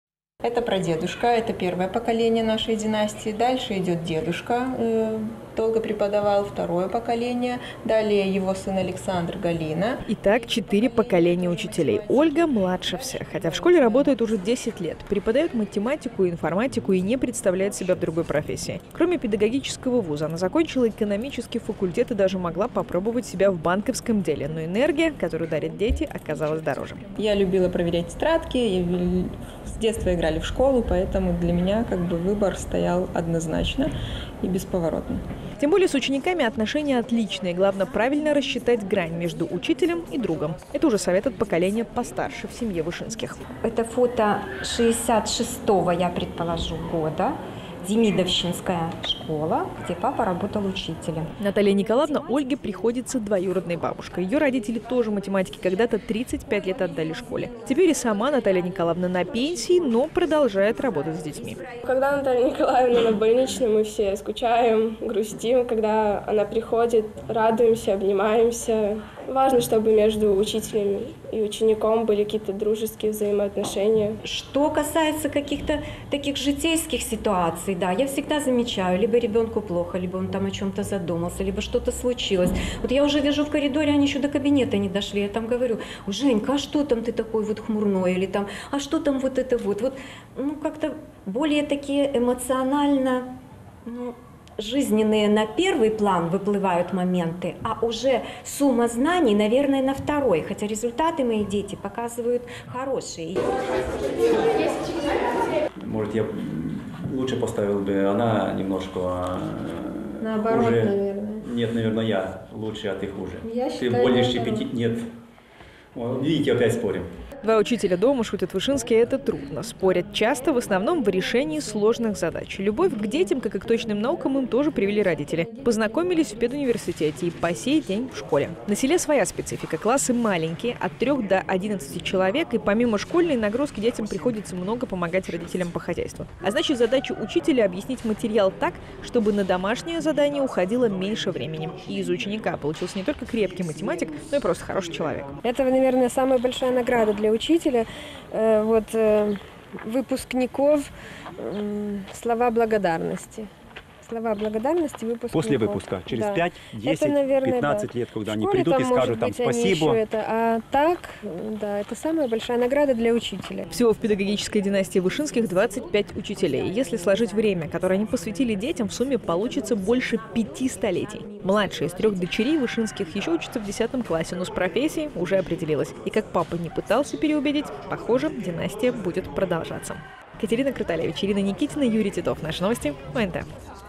Когда профессия учителя становится семейной традицией?
Династия педагогов с общим стажем более 500 лет – это  история семьи Вышинских. В начале учебного года нам удалось поговорить с представителями семьи, пять поколений которой выбирали в качестве дела жизни школу.